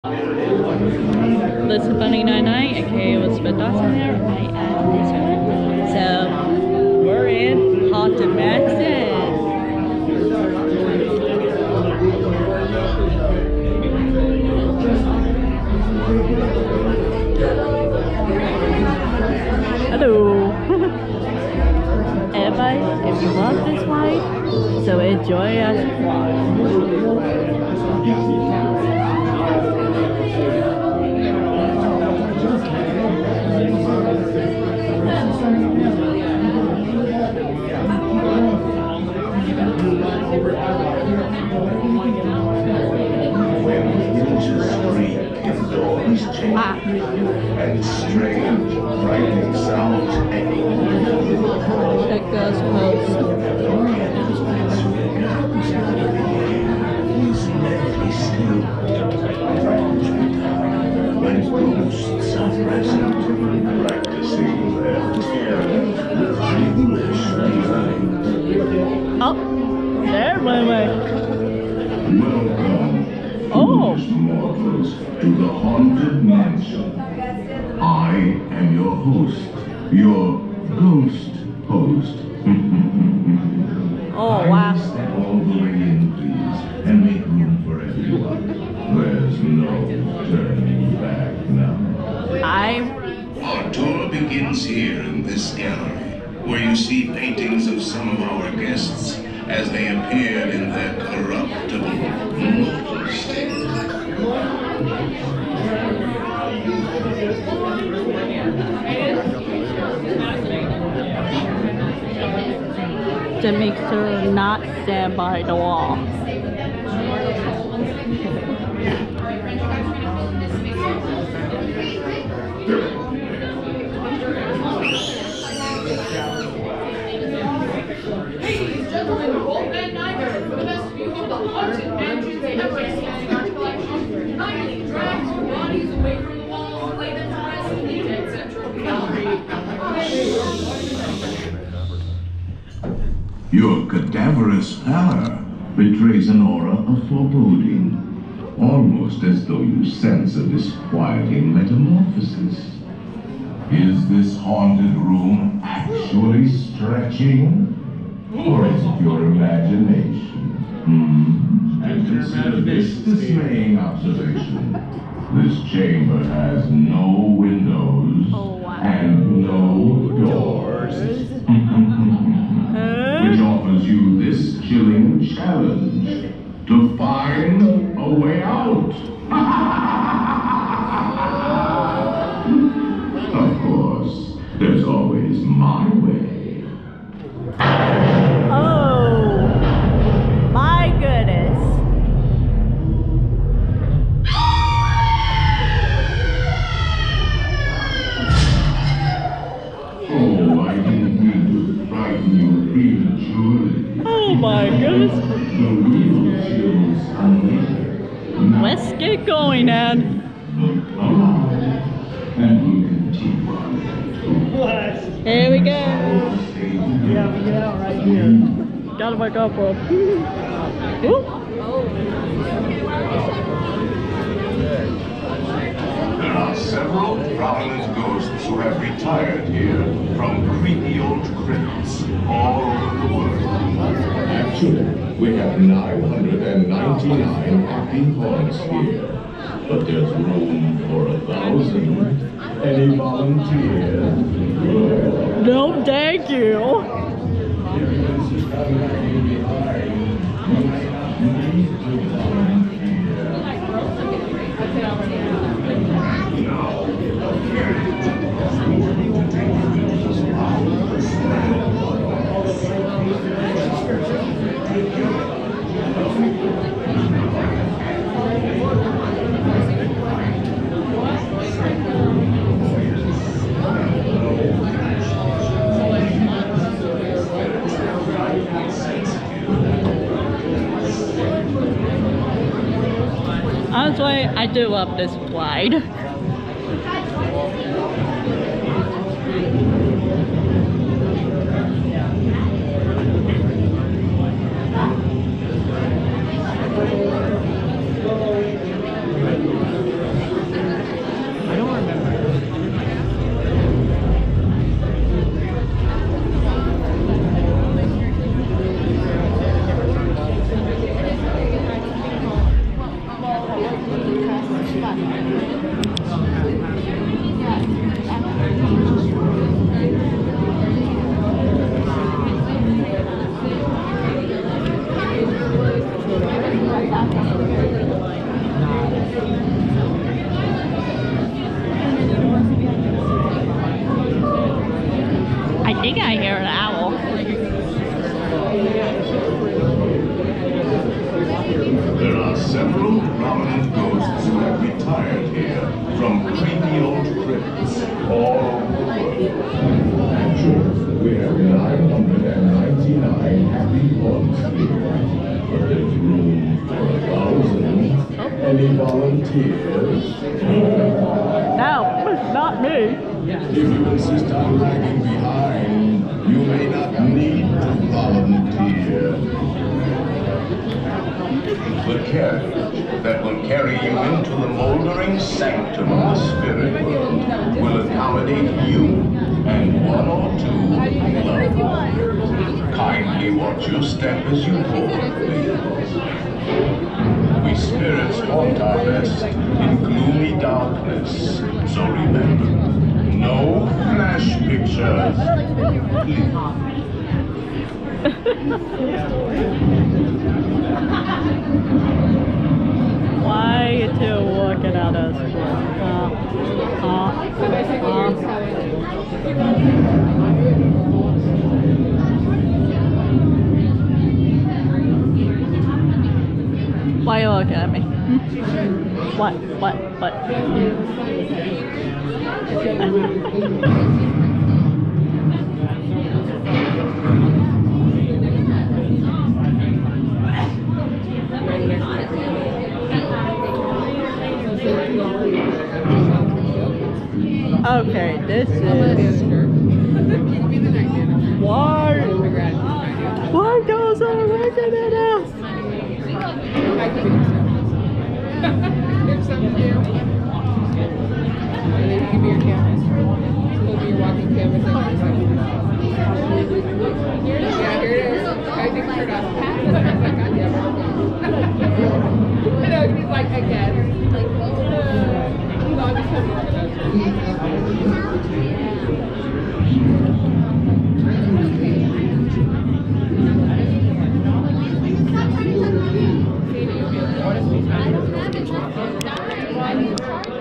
Haunted Mansion pt 1 Disneyland resort
This ride is one of my favorite rides since I was in my preteen of 2011
please enjoy this video and I know this ride changes a lot as well
this video film on May 27,2023